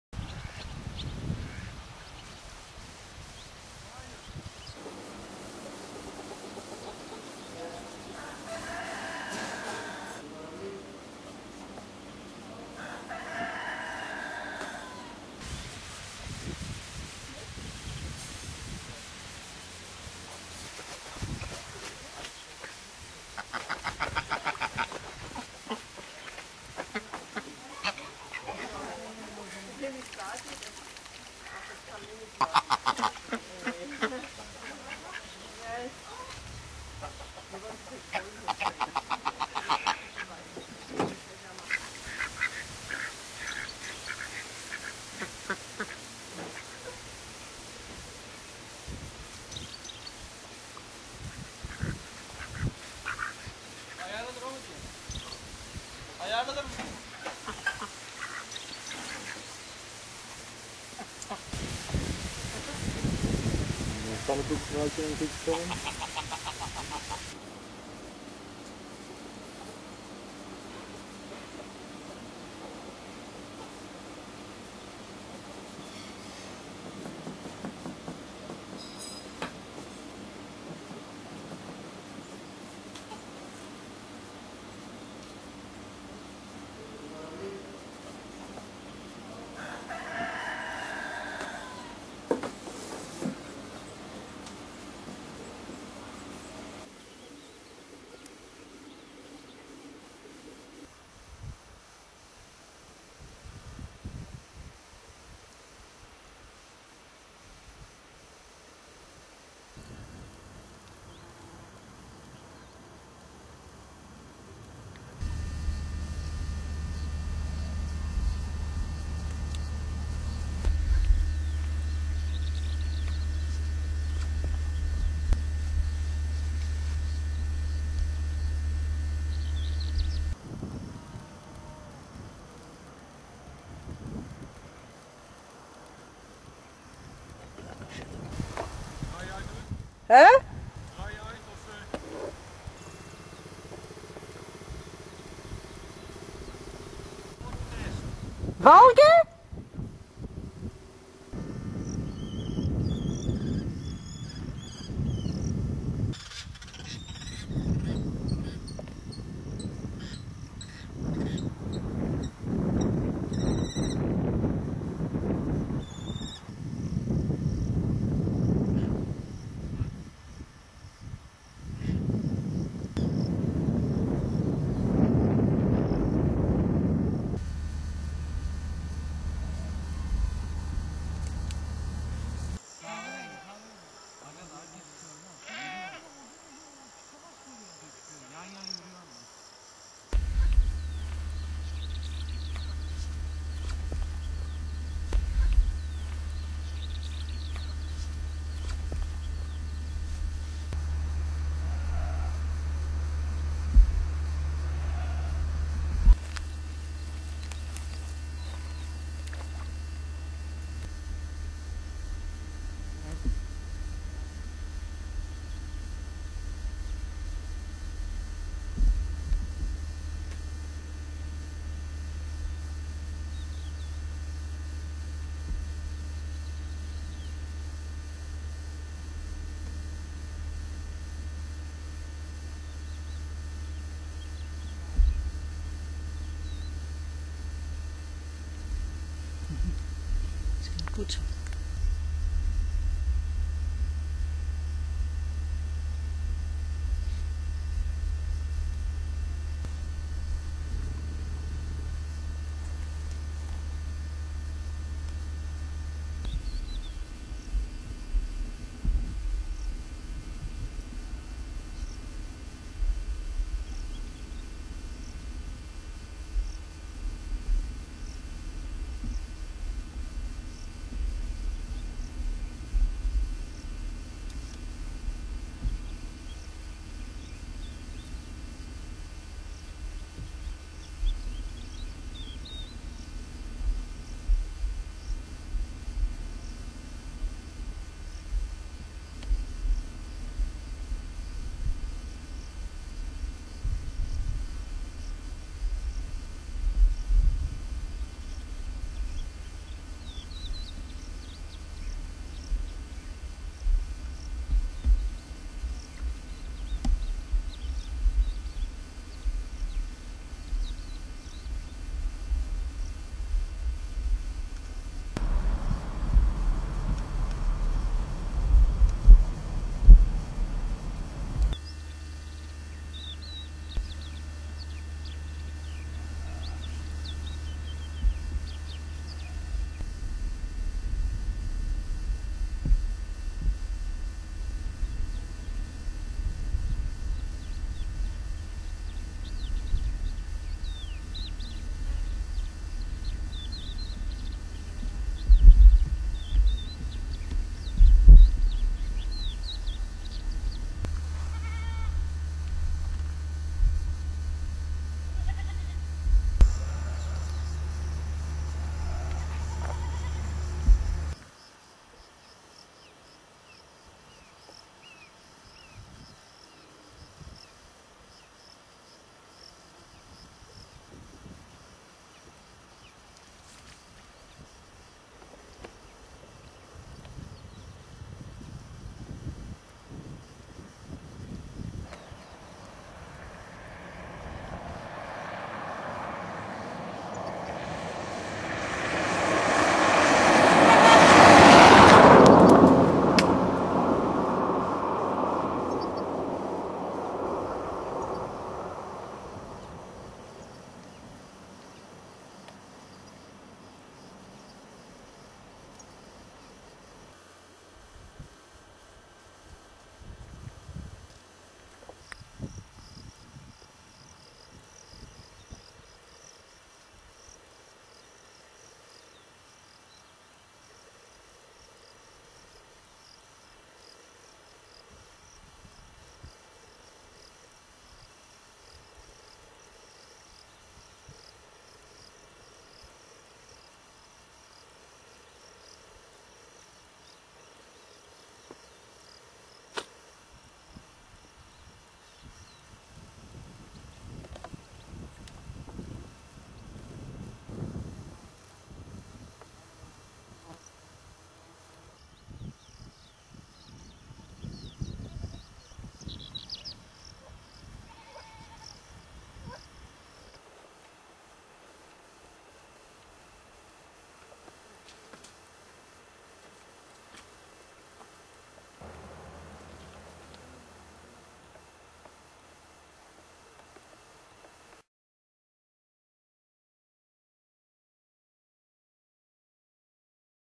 Strolling around Gölbaşı
On the way from Dalyan to İztuzu beach is a restaurant called Gölbaşı, which has a creek ending up at Sülüngür lake. Both at the restaurant itself and at the opposite side of the road you can enjoy nature at its best. Amongst others, this video shows the European goldfinch in the reeds at Gölbaşı, nesting falcons, tree frogs, storks, dragonflies and a mantis.